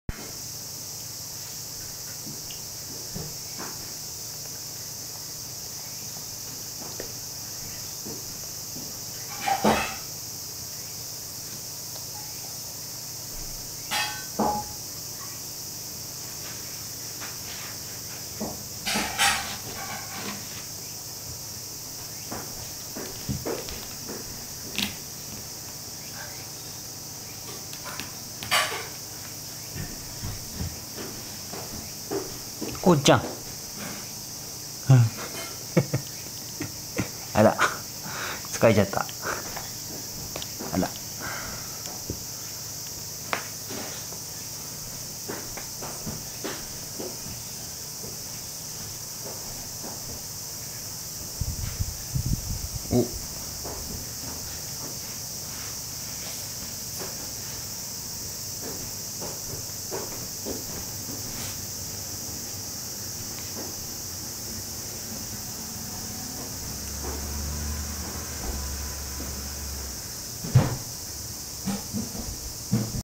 パパの実家2日目
いつも一緒のママがいませんが今日１日、おりこうさんでした。
パパは次男と平和な１日を過ごしました。